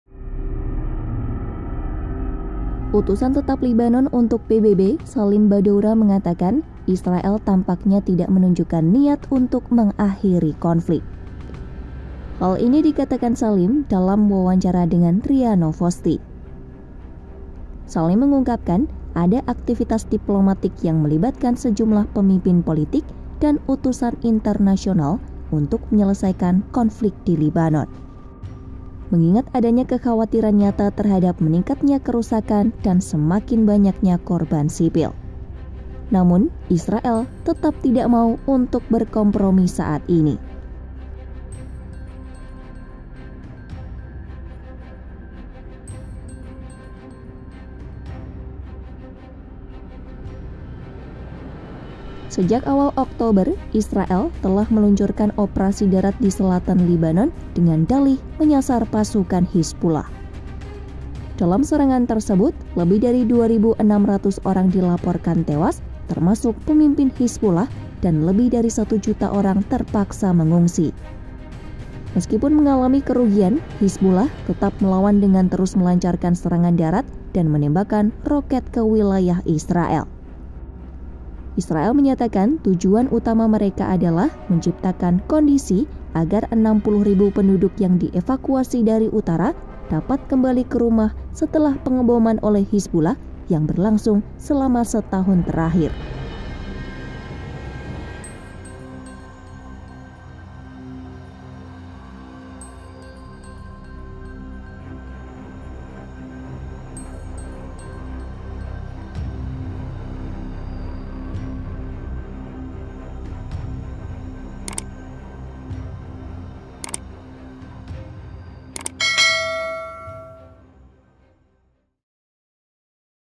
Utusan Lebanon untuk PBB Blak-blakan Ungkap Israel Tak Mau Akhiri Konflik!
Utusan tetap Lebanon untuk PBB di Jenewa Salim Baddoura mengatakan banyak opsi dibahas untuk menyelesaikan konflik di Lebanon, namun Israel sepertinya tidak ingin membuat kompromi apapun, dalam wawancara dengan RIA Novosti.

"Ada aktivitas diplomatik, melibatkan beberapa pemimpin politik dan utusan diplomatik, karena terdapat kekhawatiran nyata mengenai meningkatnya kerusakan dan jatuhnya korban sipil, serta kemungkinan meluasnya perang," ujar Salim.

Penulis Naskah: Novyana Nurmita Dewi
Narator: Novyana Nurmita Dewi
Video Editor: Agung Setiawan
Produser: Dandy Bayu Bramasta